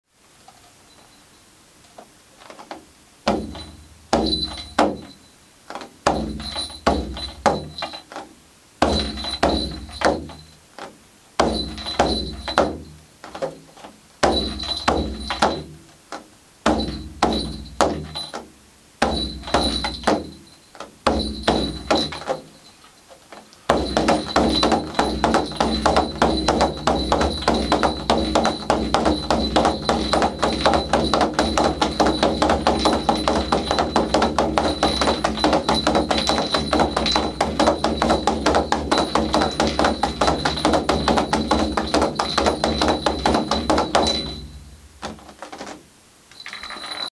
Clases de Percusión Galega: Pandeiro Cuadrado Ritmo Básico de Muiñeira Corta al Aire
Pandero Cuadrado: Ritmo Básico de Muiñeira Corta al Aire
Hay muchísimas maneras de tocar el pandero cuadrado, aquí solo se pretende mostrar una de las miles formas de tocarlo
para llevar el ritmo puedes usar la onomatopeya:

poporo poporo poporo

po= D
ro= I